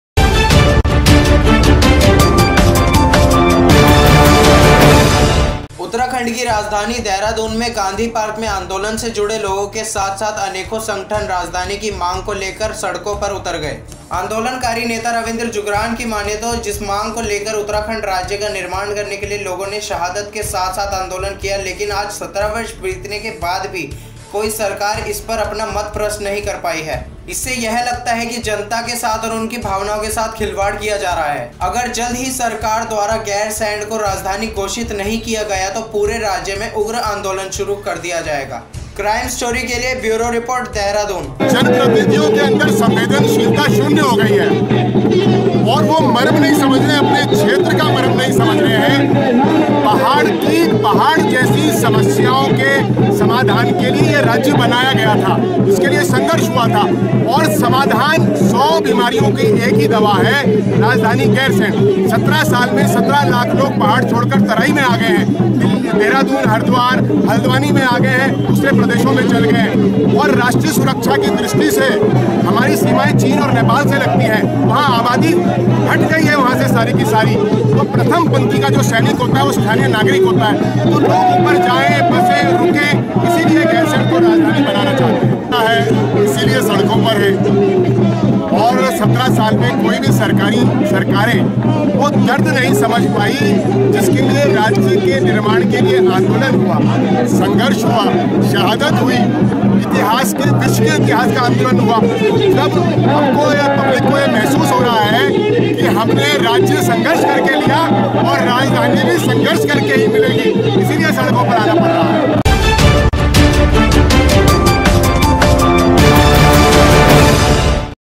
गैरसैण पर आंदोलन की तैयारी
अनेकों संगठन राजधानी की मांग को लेकर सड़कों पर उतर गए
आंदोलनकारी नेता रविंद्र जुगरान की माने तो जिस मांग को लेकर उत्तराखंड राज्य का निर्माण करने के लिए लोगों ने शहादत के साथ-साथ आंदोलन किया लेकिन  आज 17 वर्ष बीतने पर भी कोई भी सरकार इस पर अपना मत स्पष्ट नहीं कर पाई  हैं
इससे यह लगता है कि जनता के साथ और उनकी भावनाओं के साथ खिलवाड़ किया जा रहा है